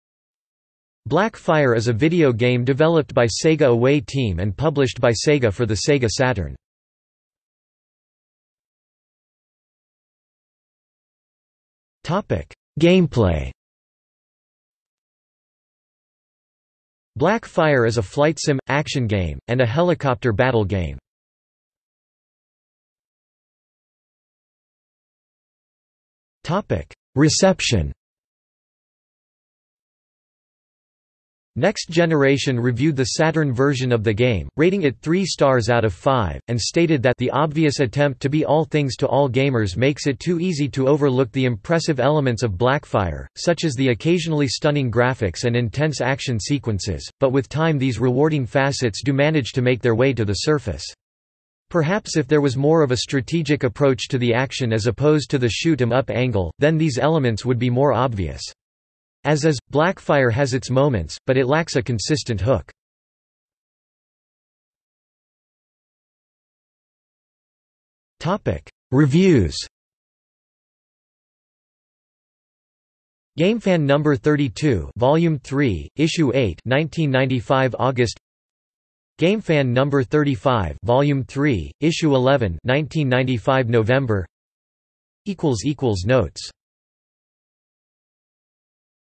Black Fire (game) | Wikipedia audio article
00:00:08 1 Gameplay
00:00:22 2 Reception
00:01:04 3 Reviews

- Socrates

SUMMARY
Black Fire is a video game developed by Sega Away Team and published by Sega for the Sega Saturn.